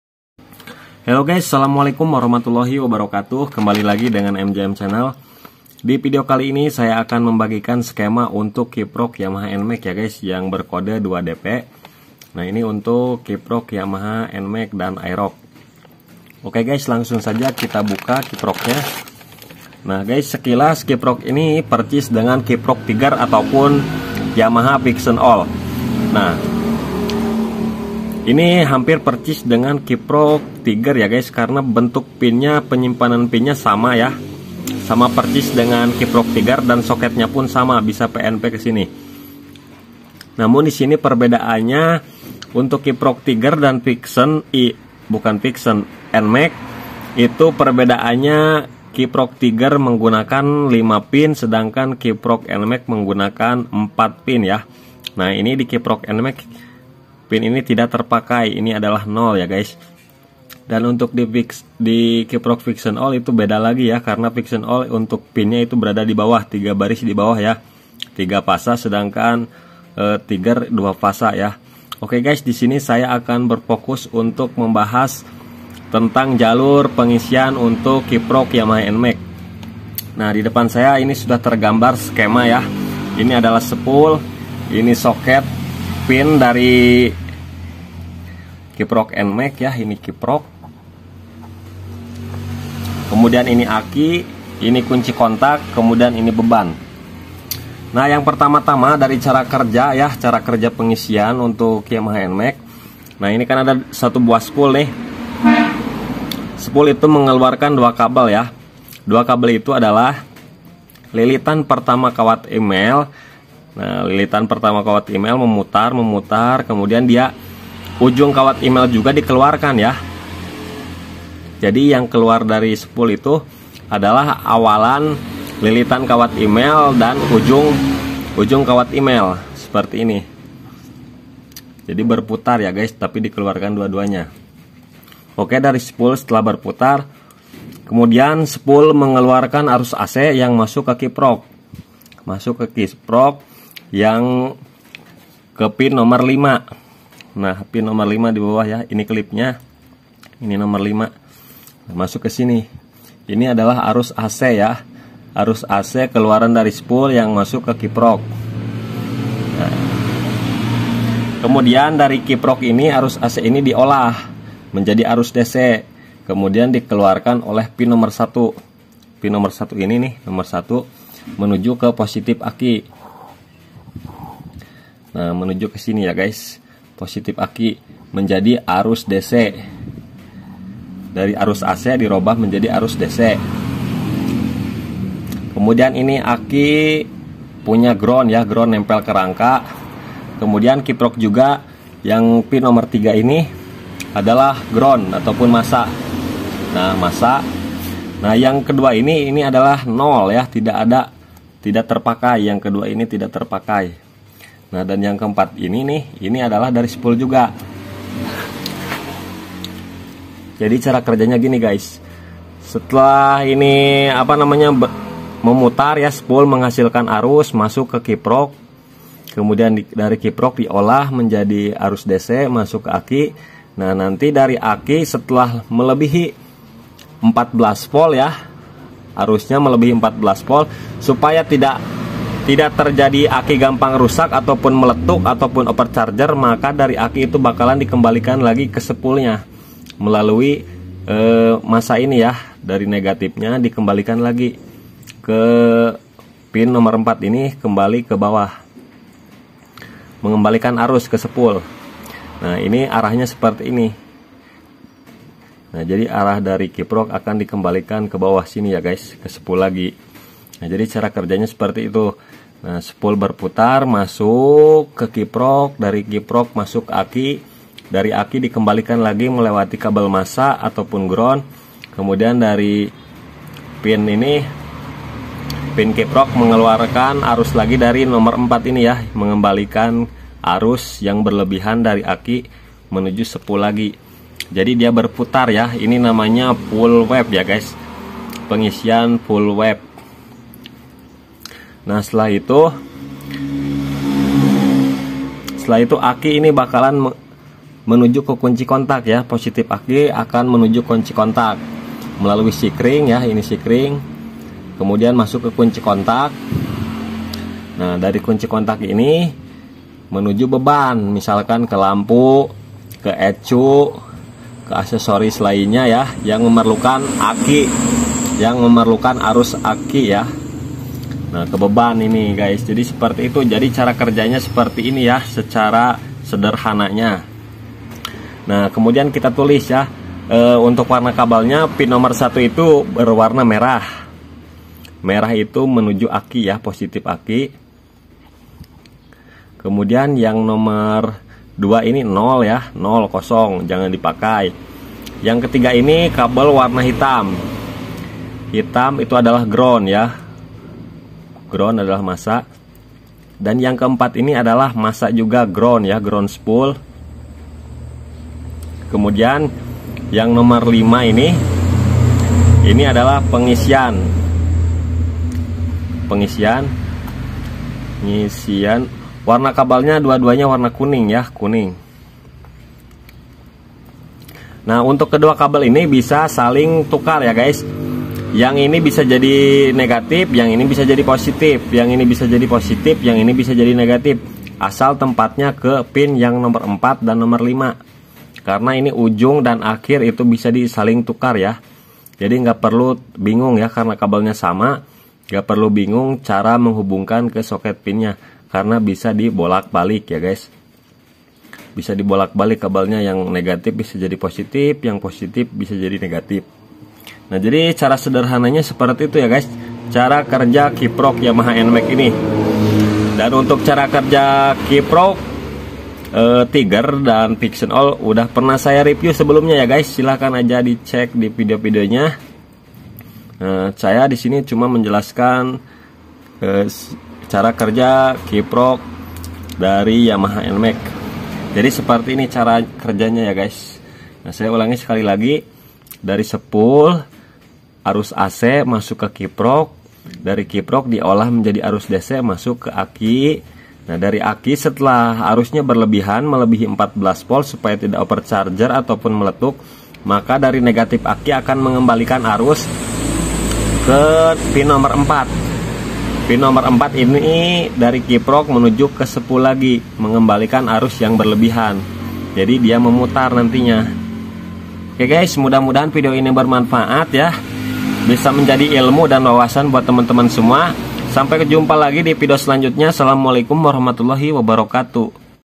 JALUR PENGISIAN KIPROK YAMAHA NMAX AEROX FULLWAFE
skema rangkaian dan cara kerja dari kiprok yamaha nmax bahas jalur kabel dan cara kerja untuk sisitem pengisian dari kiprok nmax atau aerox di mulai dari spul kiprok aki sampai ke beban

mudah mudahan video ini bermanfaat